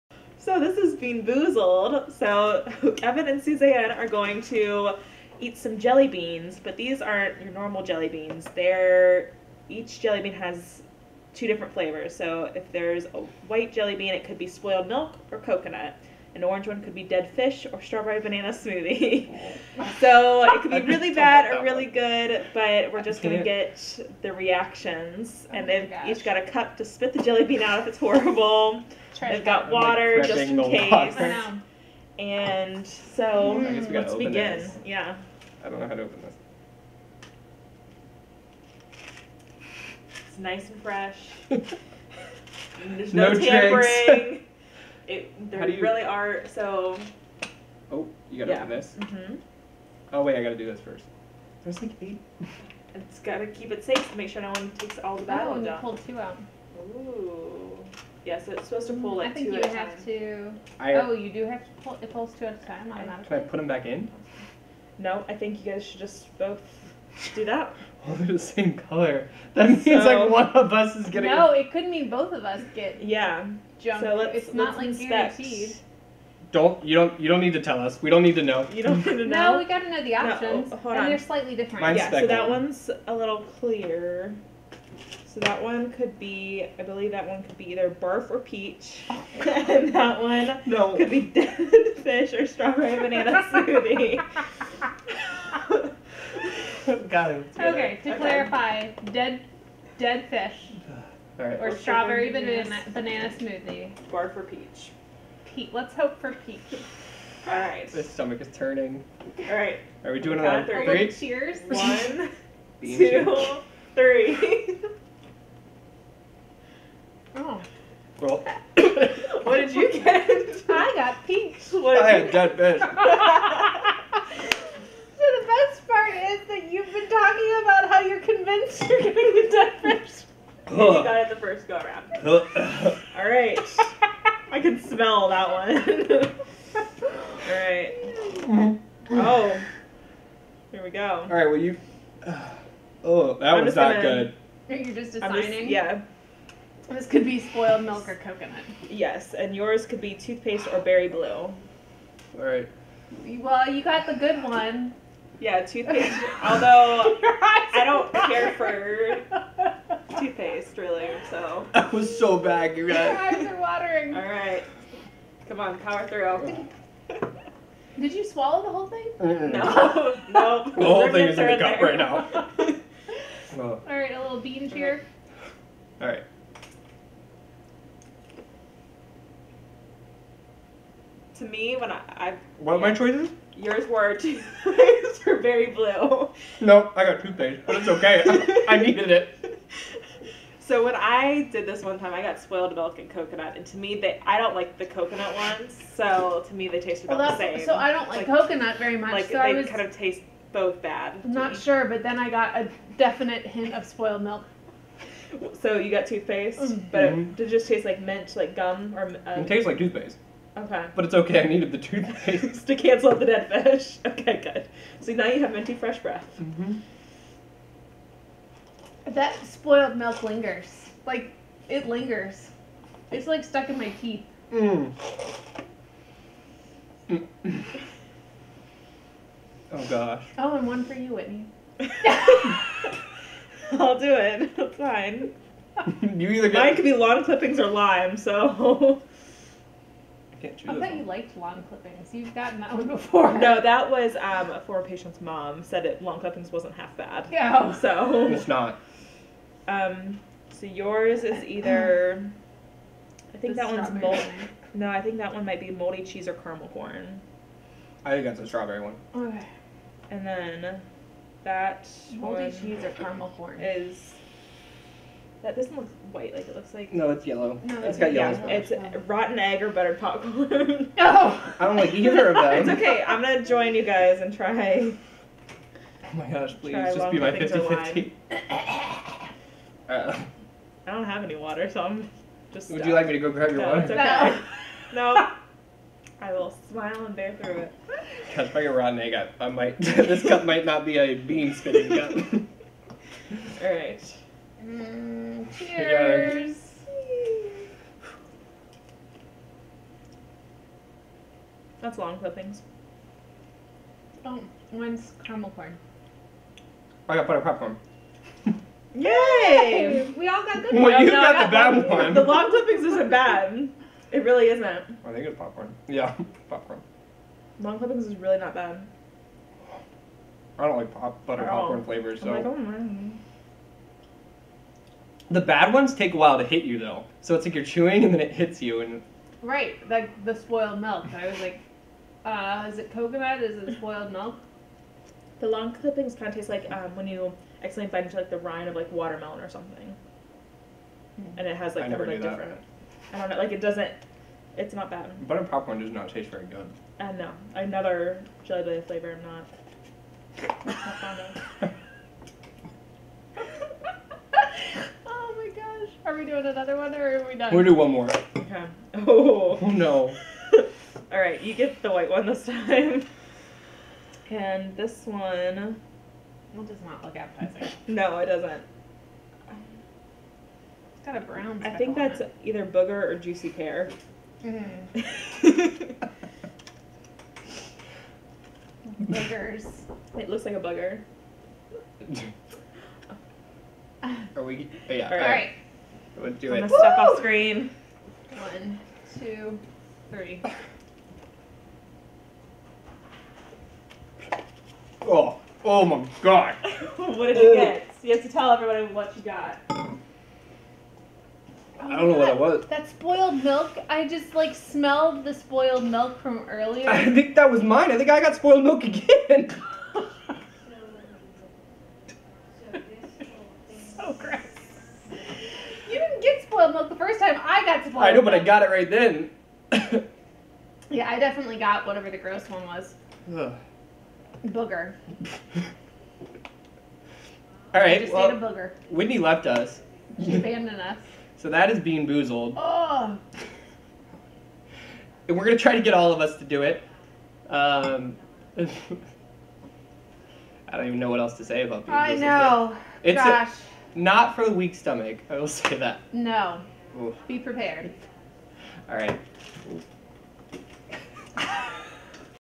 Bean Boozled with WKID 33
Suzanne and Kevin take their chances playing Bean Boozled!